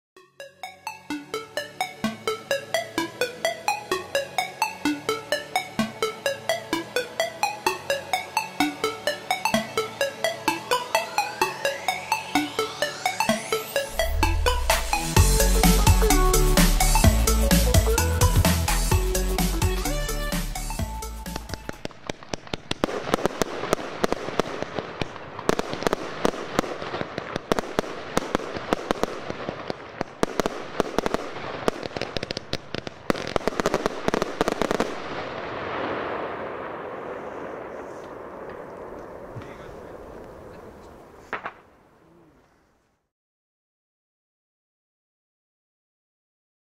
Hoops Pyrotechnik - BASTED (NEUHEIT 2018) (20mm & 80-85m weg) (UNFASSBAR!)
Alter Falter was da raus kommt...Einfach Unfassbar!Ich stand 80-85m weit weg!Das Teil hat nur 20mm!Heftig!Zählte für mich mit zu den absoluten Highlights an Silvester!

Danke an Hoops Pyrotechnik!

Hier die Daten:

Kaliber - 20mm
Kategorie - F2 (Silvester Feuerwerk)
Lager Gruppe - 1.4G
Film Entfernung - 80-85 Meter (Abgemessen!)
Preis - 32 Euro
Meine Bewertung